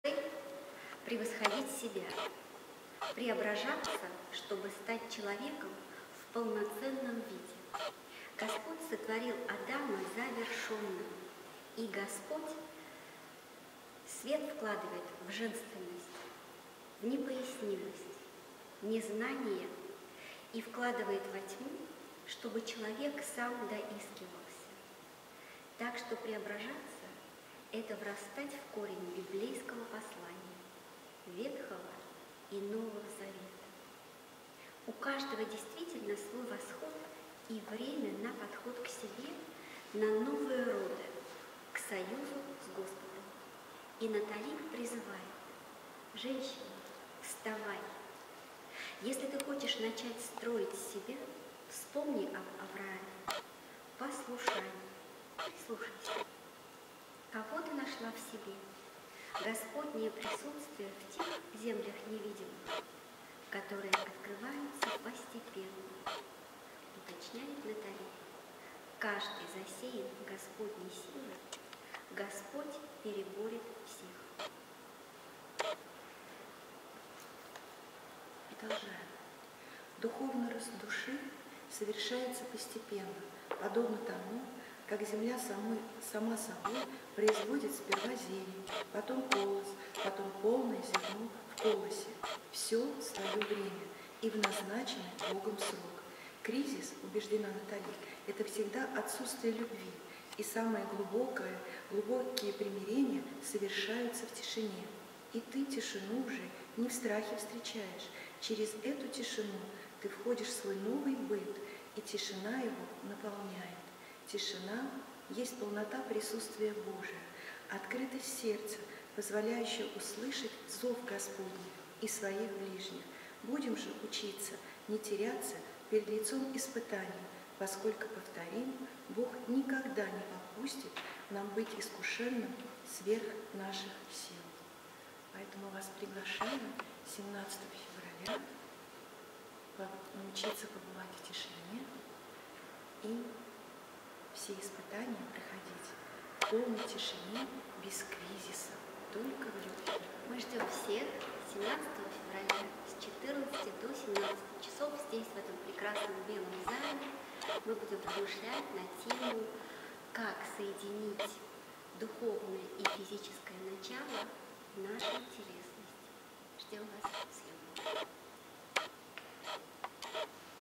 Приглашение на Встречу, посвященную Дню Памяти Натали Дроэн 17 февраля 2021 года
Предлагаем Вашему вниманию запись нашей Встречи. Самое главное то, что в ней есть и размышления на самые насущные женские вопросы, и практические занятия, где акцент делается на поиске лёгкости движения и поиске дыхания, когда возможно словно бы одухотворить плоть, продолжая исследовать «по себе» тот бесценный опыт соединения духовного и физического начала, который заповедала всем женщинам наша удивительно цельная подвижница Натали.

Встреча состоялась 17 февраля 2021 года в Санкт-Петербурге в большом белом зале комплекса Спорт Палас на Крестовском острове.

Общая продолжительность видео: 2 часа 1 минута (121 минута).